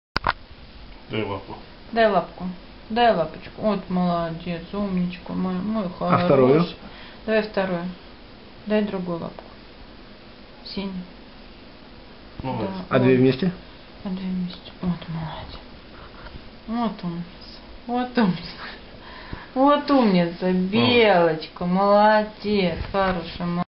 Trick-doing cat
Who said that Persian cats were lazy and did nothing but sleep all day?